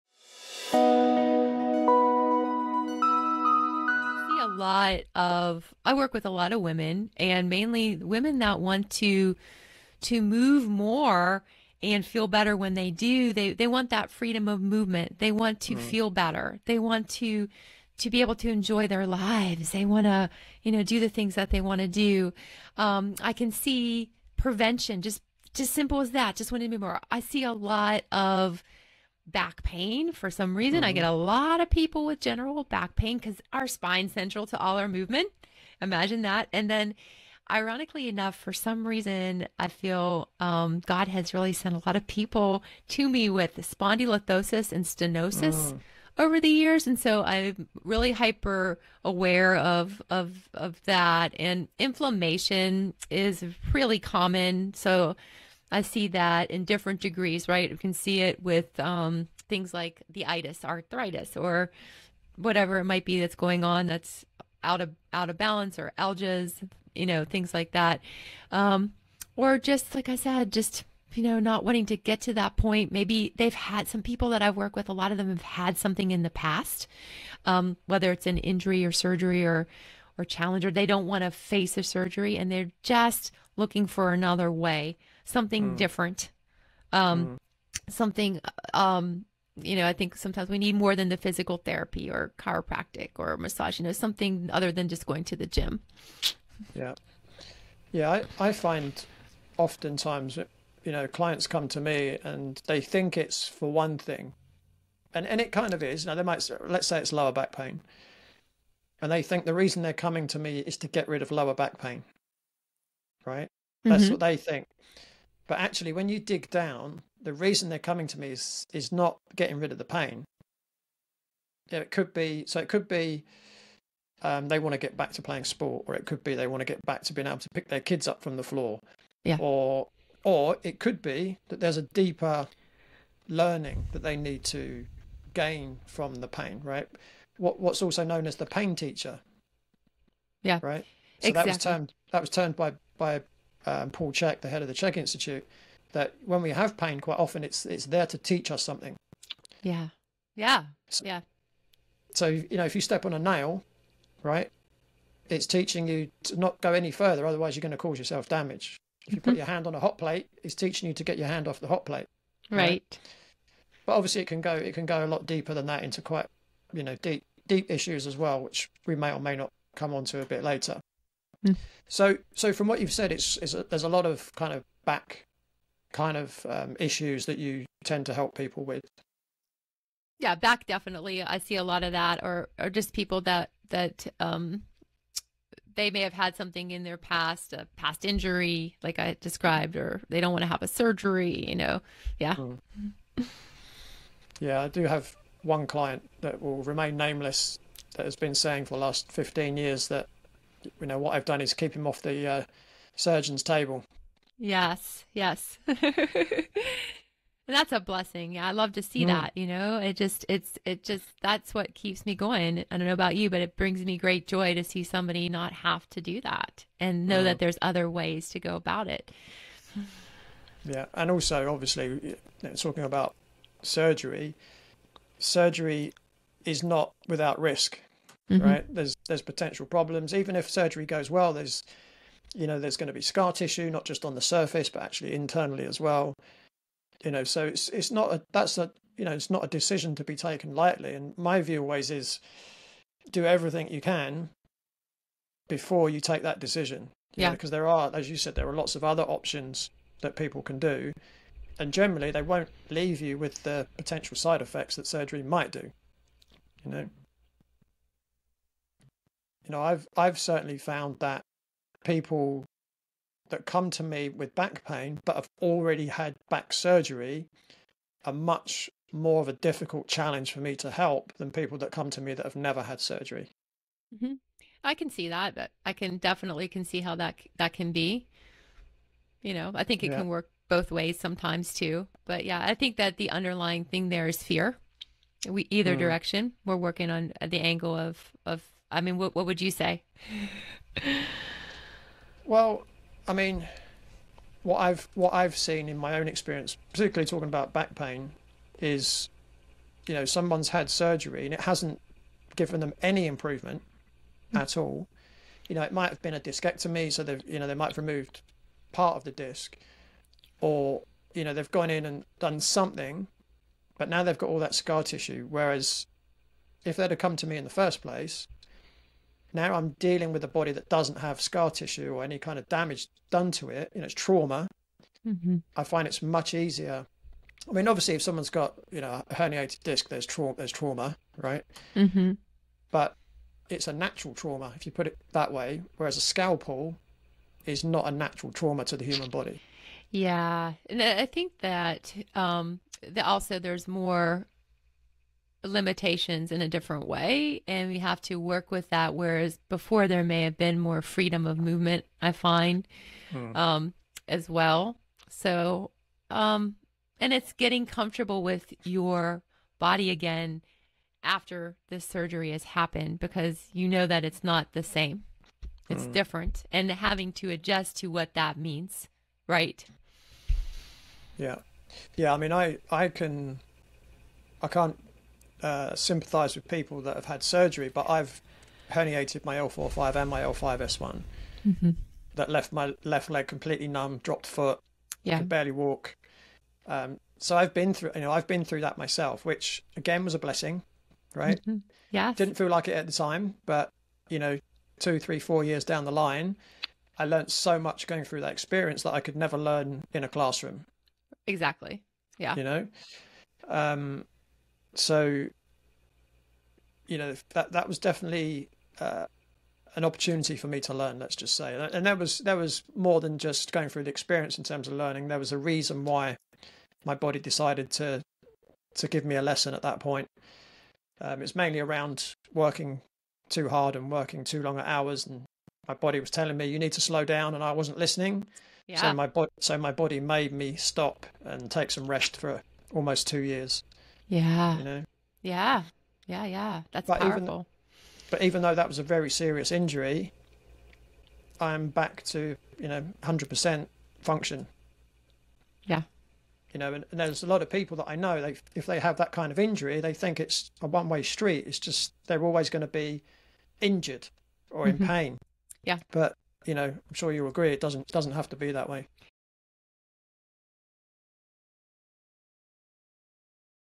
Common Pain Conditions with Cari Vann
In this episode, I spoke with Cari Vann, Exercise Physiologist & Holistic Movement Mentor with over 26 years of experience both professionally and personally. Cari has always loved to move and struggled with pain from an early age, which motivated her to find solutions. This led Cari on a journey to where she is today, helping women overcoming pain using a holistic approach.

We discussed:

Cari's background
Common pain conditions Cari sees in her practise
Key elements to pain prevention
The 'Complete Well Body Method'
Key considerations for eliminating back pain
Keys to overall health & wellness

Don't forget to leave a Rating for the podcast!